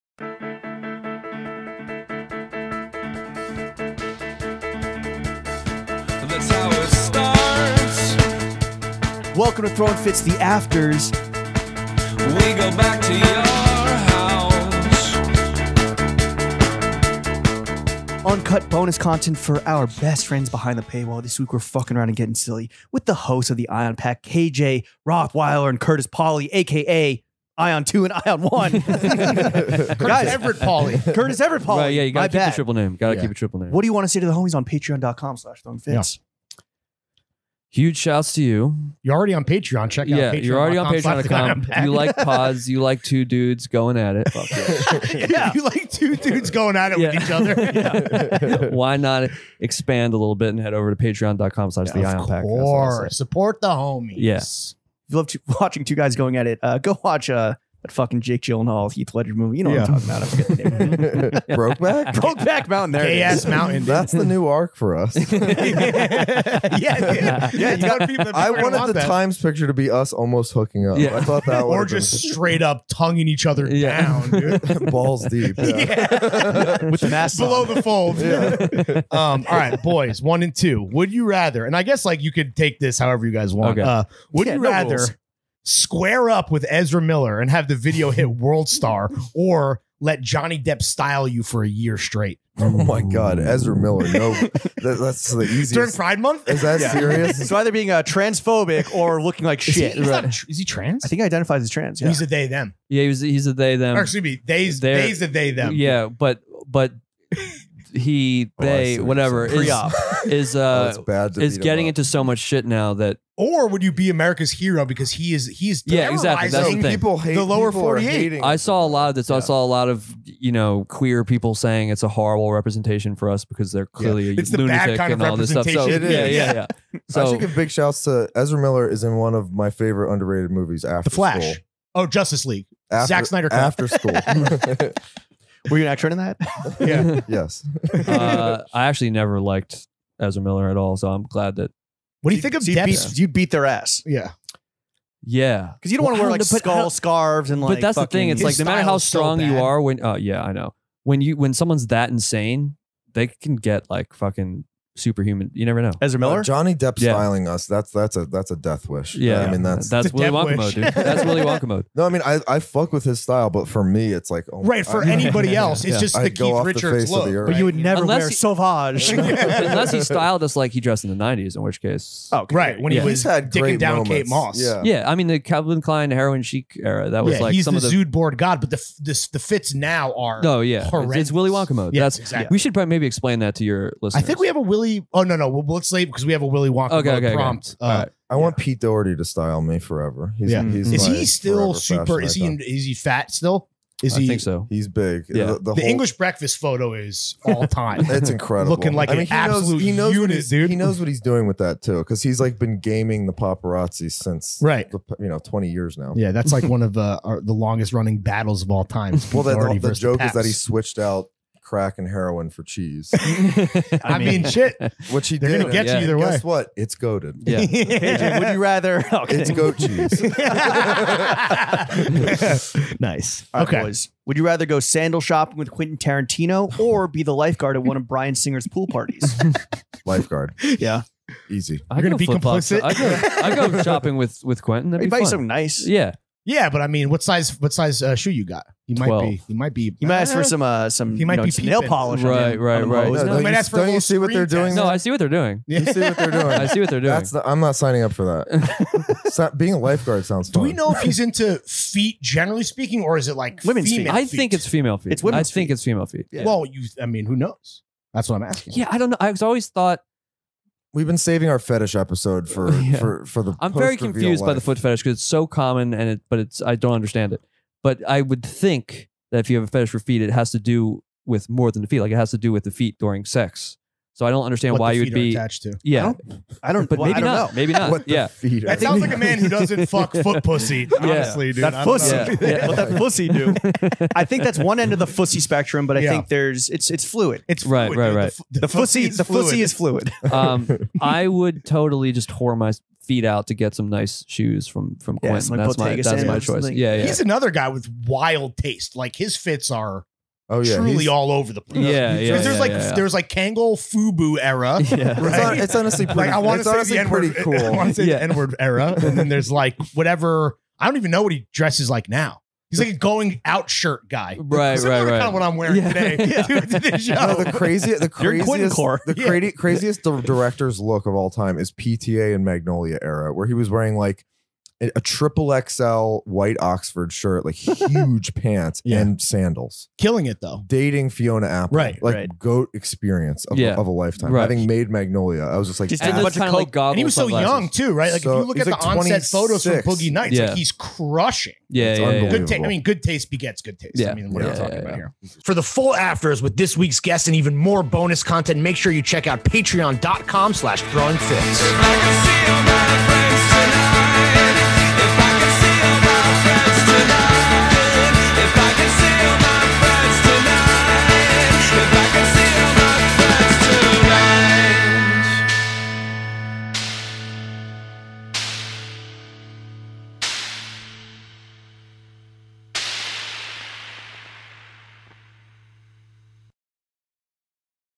*PATREON PREVIEW* The Afters with The Ion Pack
On our new weekly lightning round mini ep with Curtis Everett Pawley and KJ Rothweiler, co-hosts of The Ion Pack podcast, we’re fucking around with squaring up with Ezra Miller, getting styled by Johnny Depp, sandal shopping with Quentin Tarantino, lifeguarding Bryan Singer’s pool party, going Willy Wonka Mode, cutting off a toe, being in the same room while your boy smashes, having zero communication with your boy for a year, speaking solely in dialogue from Crash (2006), only watching silent films, Freaky Friday scenarios with each other and much more.